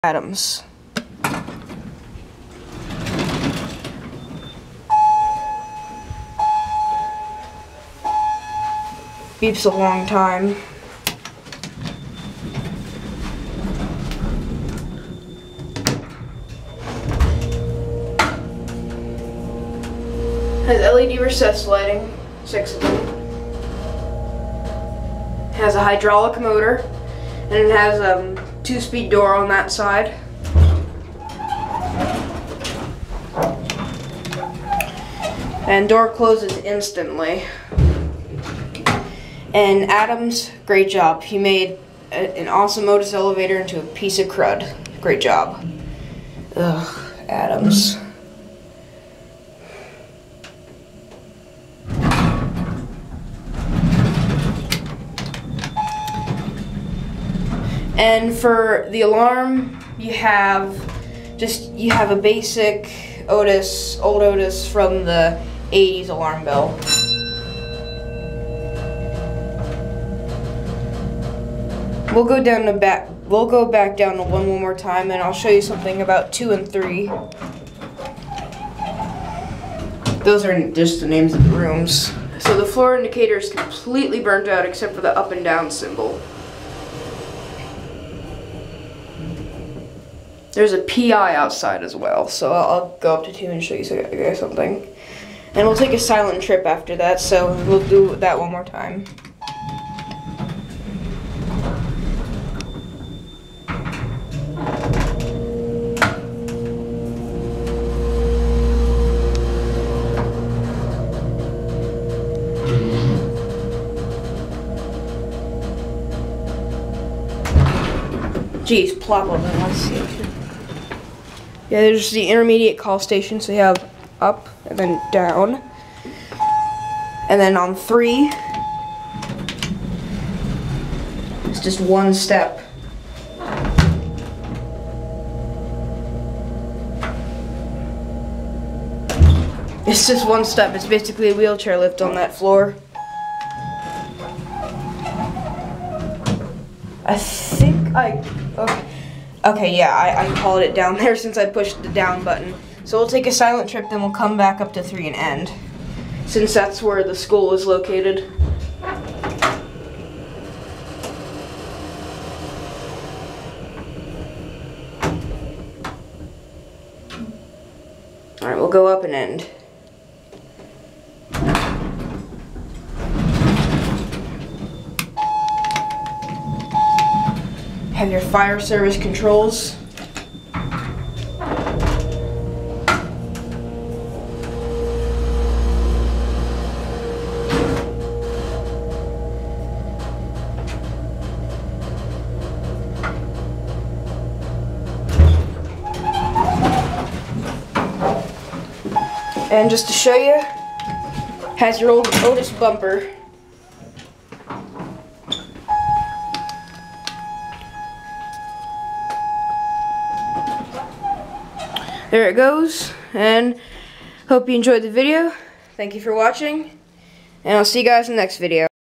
OTIS Hydraulic Elevator - Baltimore Hebrew Congregation - Pikesville, MD | ECFE
For clarity, I am not Jewish. My old elementary and middle school rented space here. © Elevator and Ceiling Fan Enthusiasm.